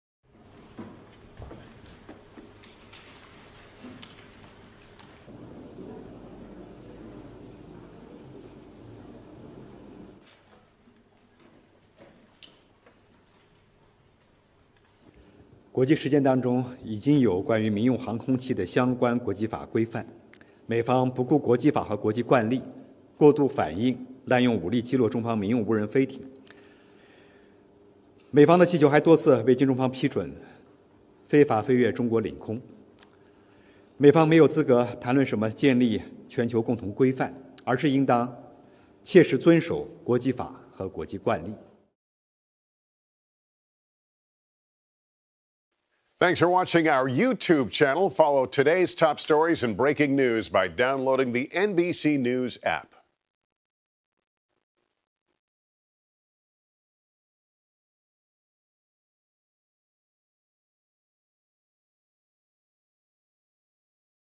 China says U.S. downing suspected spy balloon 'disregarded international law'
China’s foreign ministry said the U.S. “disregarded international law” when it shot down a suspected Chinese spy balloon that entered U.S. airspace. China’s foreign ministry spokesperson Wang Wenbin went on to say that balloons from the U.S. have flown “illegally over Chinese airspace without Chinese approval.”